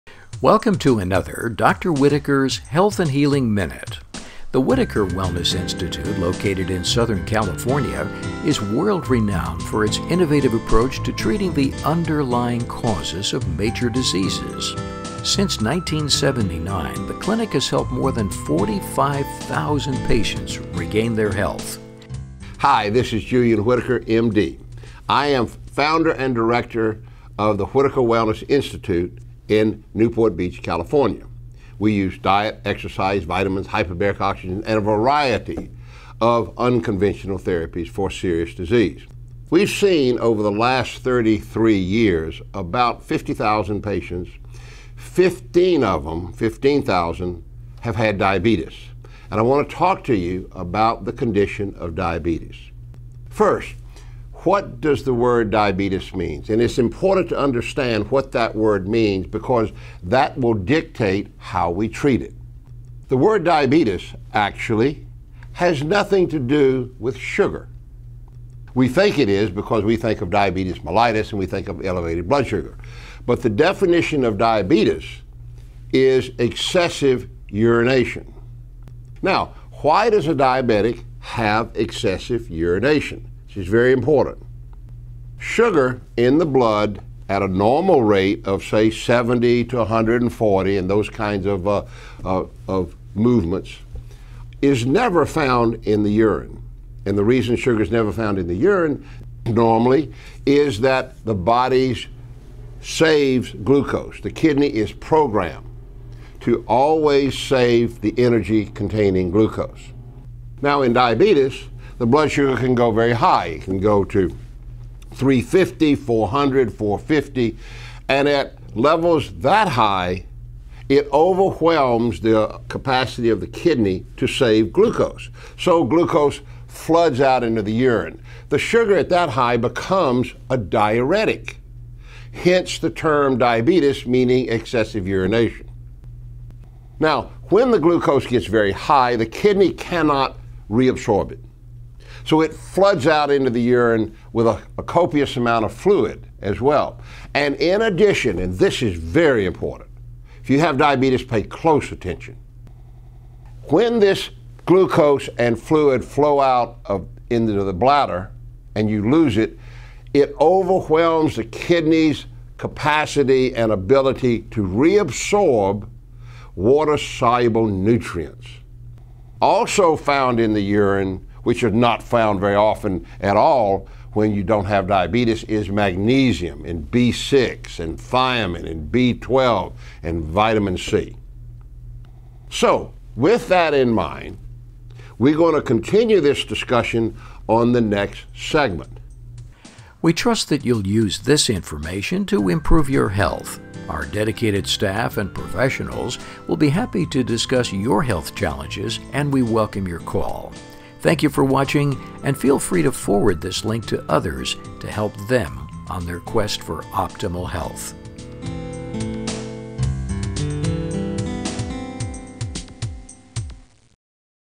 Dr. Whitaker explains, "What is Diabetes?"
Dr. Whitaker explains the basics of diabetes and how you can reverse it. Dr. Julian Whitaker is America's alternative medicine doctor, author of 13 best-selling health books and founder of the Whitaker Wellness Institute in Newport Beach, California, where thousands have been successfully treated since 1979.